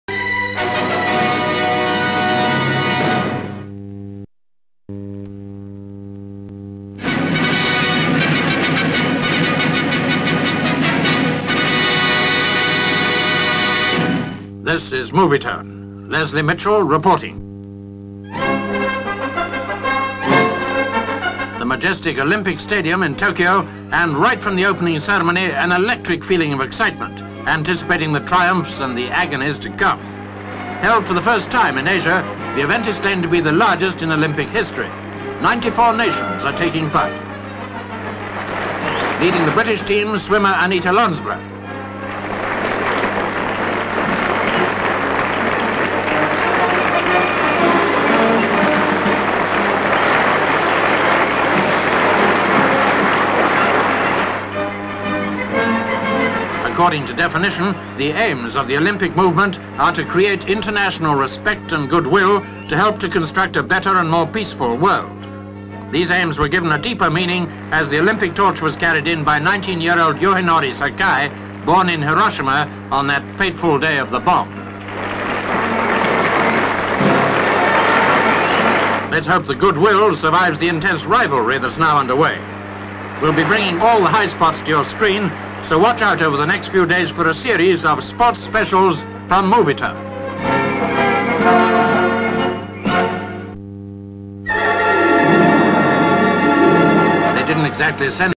OLYMPICS 1964
(15 Oct 1964) This year the Olympic Games are held for the first time in Asia. They are claimed to be the largest in Olympic history. Ninety four Nations are taking part.